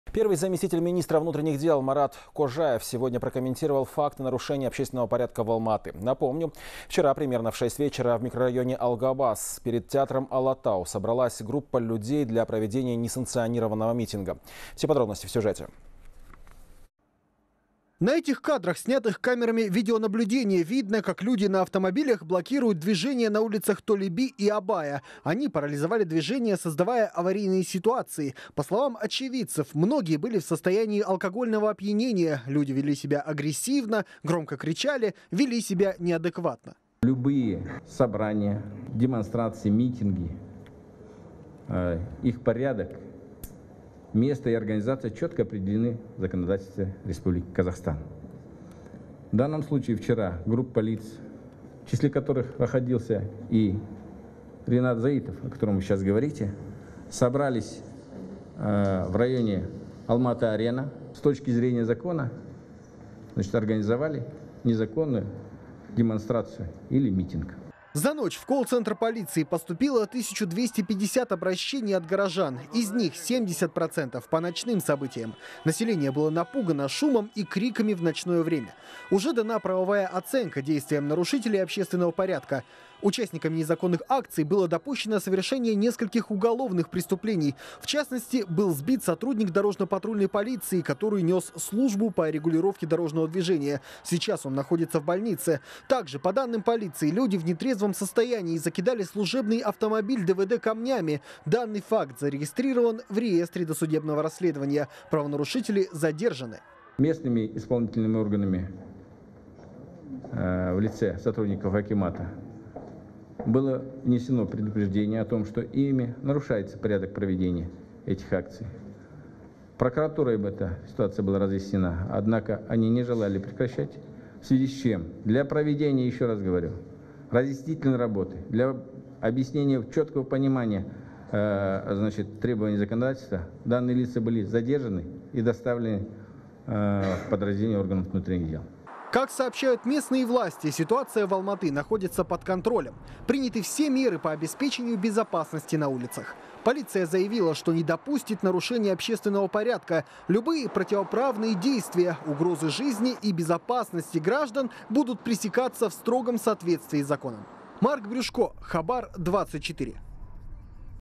В МВД прокомментировали нарушения общественного порядка в Алматы
Первый заместитель министра внутренних дел Марат Кожаев прокомментировал факты нарушения общественного порядка в Алматы, сообщает «Хабар 24».

Напомним, вчера примерно в шесть вечера в микрорайоне «Алгабас» перед театром «Алатау» собралась группа людей для проведения несанкционированного митинга.

На кадрах, снятых камерами видеонаблюдения, видно, как люди на автомобилях блокируют движение на улицах Толе би и Абая. Они парализовали движение, создавая аварийные ситуации. По словам очевидцев, многие были в состоянии алкогольного опьянения, люди вели себя агрессивно, громко кричали, вели себя неадекватно.